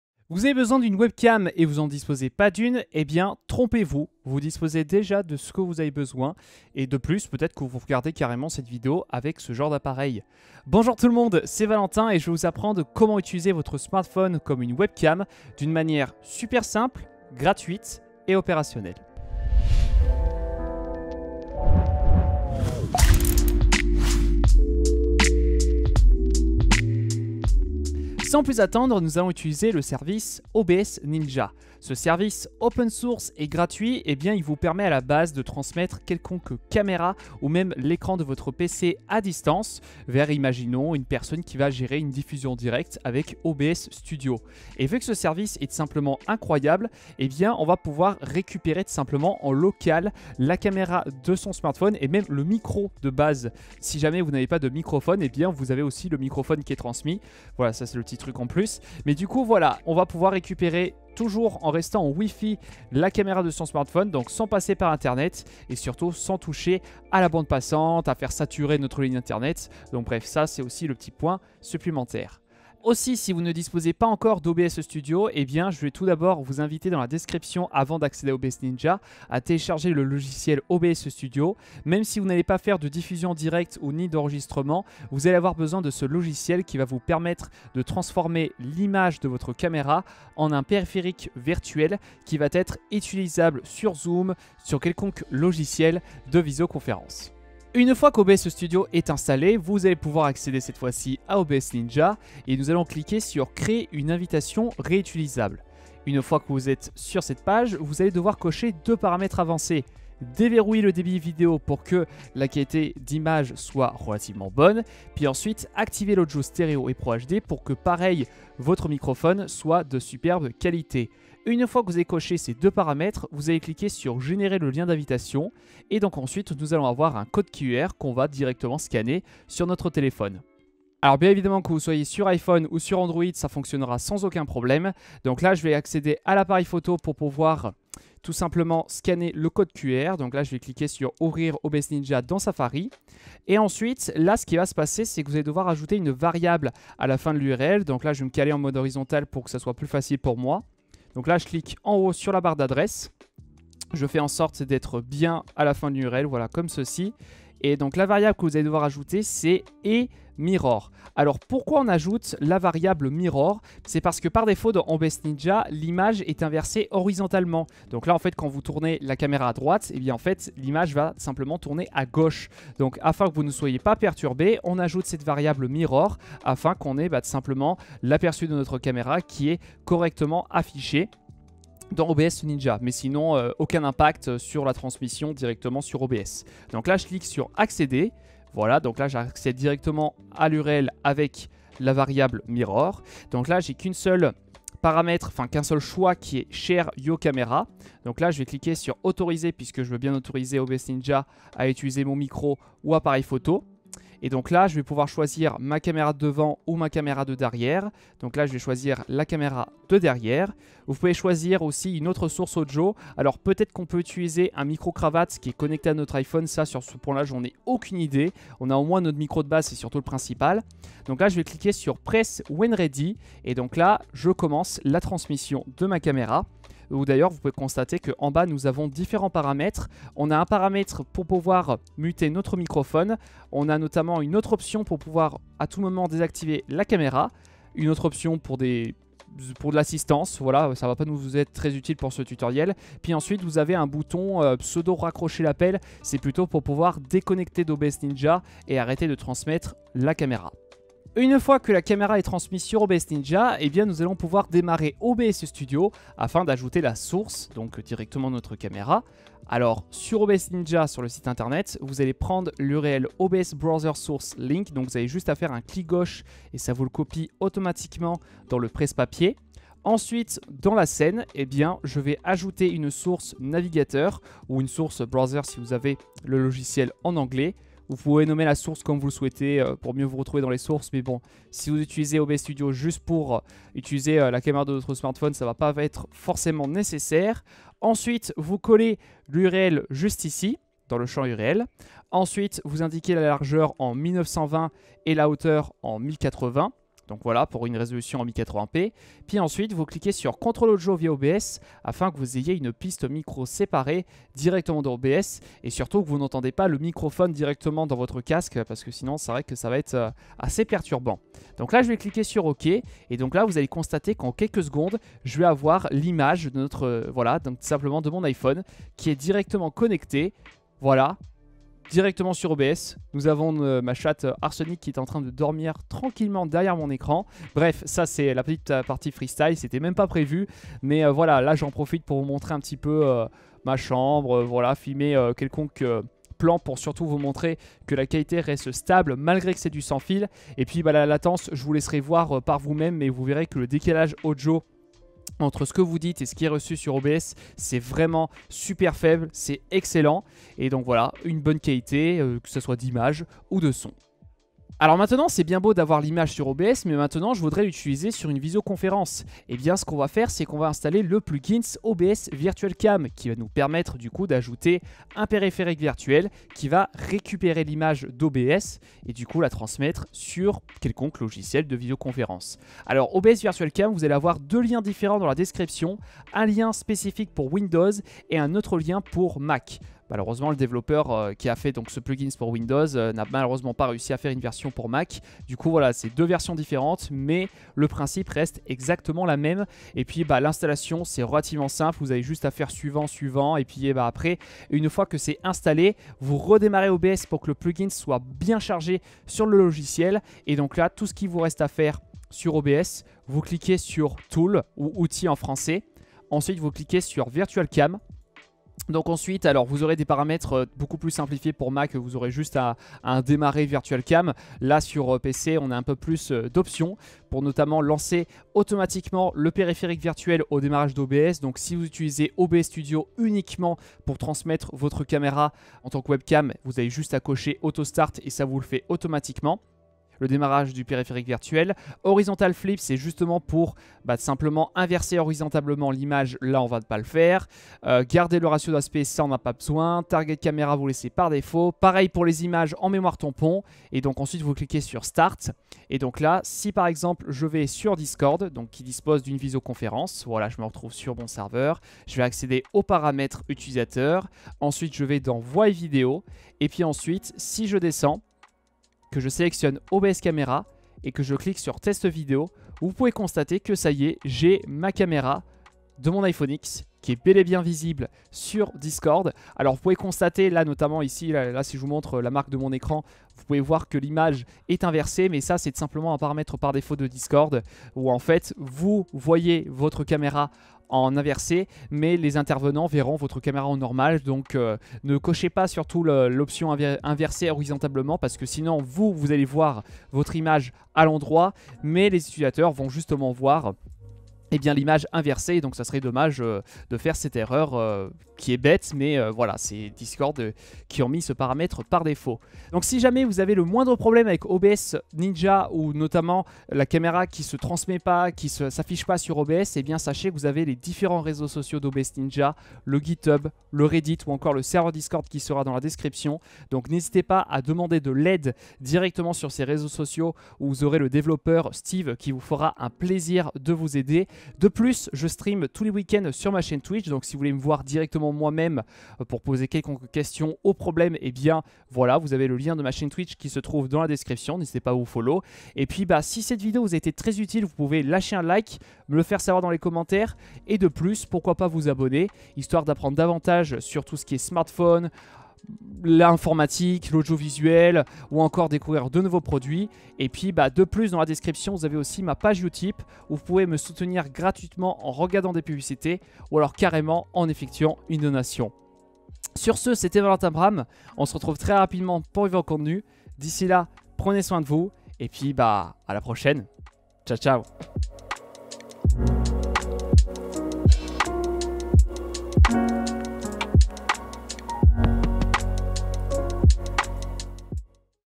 Utiliser son smartphone comme webcam (2020)
Cette vidéo vous apprend comment utiliser son smartphone comme webcam gratuitement, en wifi et sur n'importe quel logiciel.

Horodatage :
00:00 - Introduction
01:38 - Configurer le smartphone
04:11 - Récupérer l'image sur OBS
05:55 - Utilisation d'OBS VritualCam
08:23 - Fin